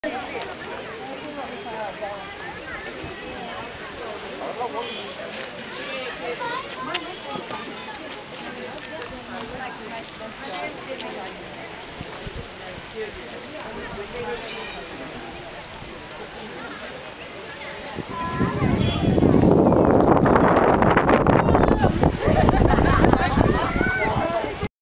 Monkeys running around
A little film of monkeys at Nandi Hills